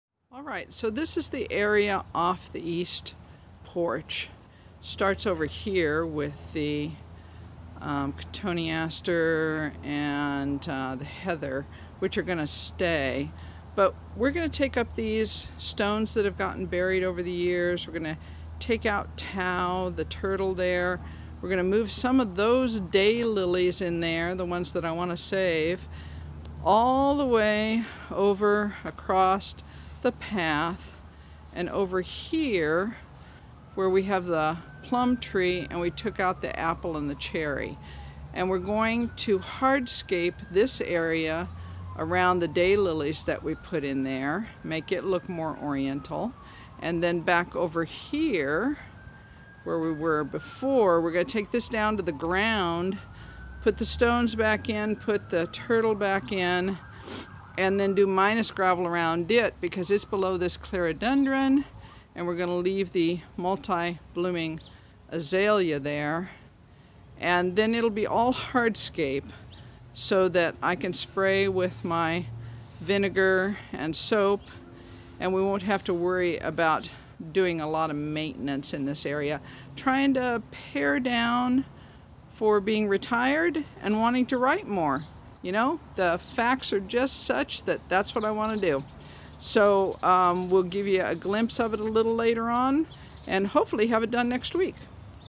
Our Quiet Corner - Spring Project 2018 - Part 1
A facelift with new hardscape in the East garden